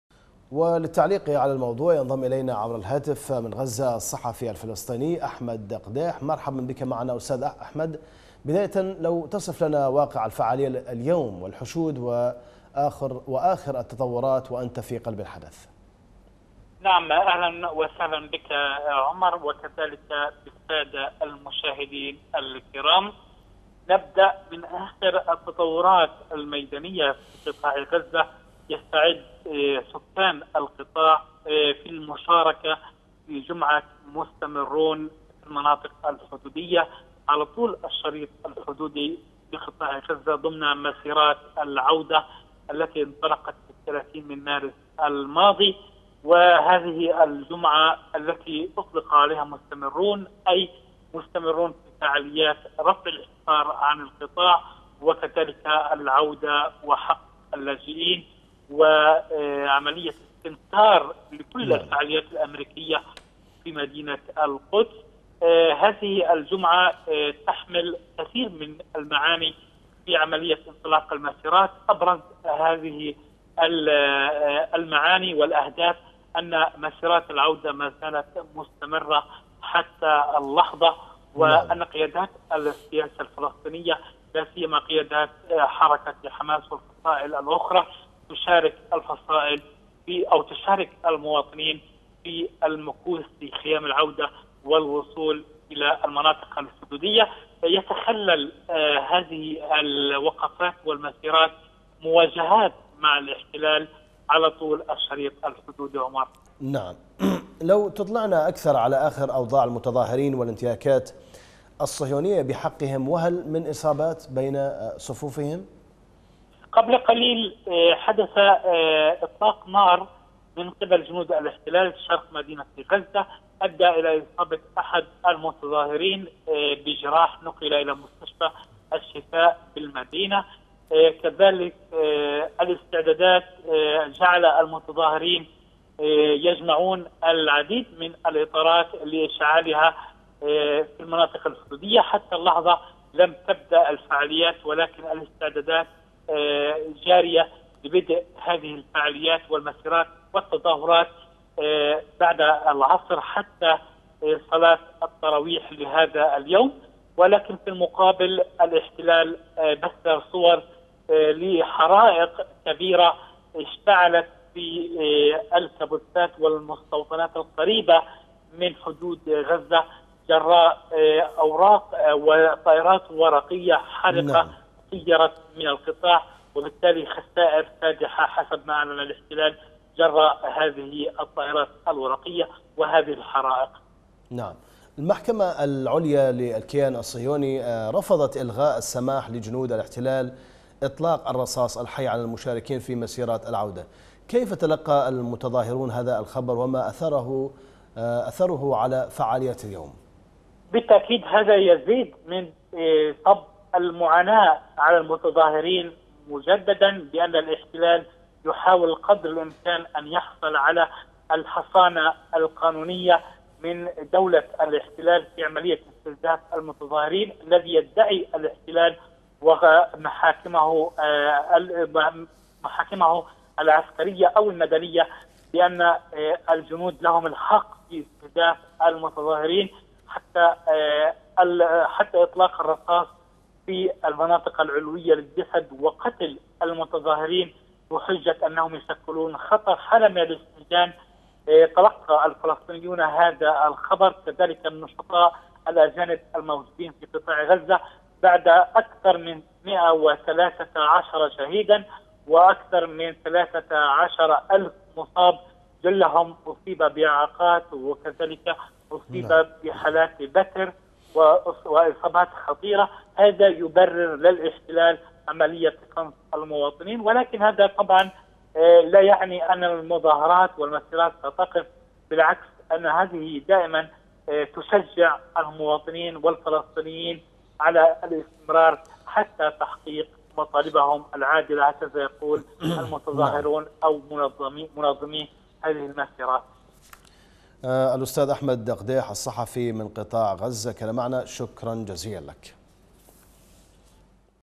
مداخلة أحمد قديح بشأن تواصل فعاليات مسيرة_العودة_الكبرى للجمعة التاسعة على التوالي
مداخلة مداخلة أحمد قديح بشأن تواصل فعاليات مسيرة_العودة_الكبرى للجمعة التاسعة على التوالي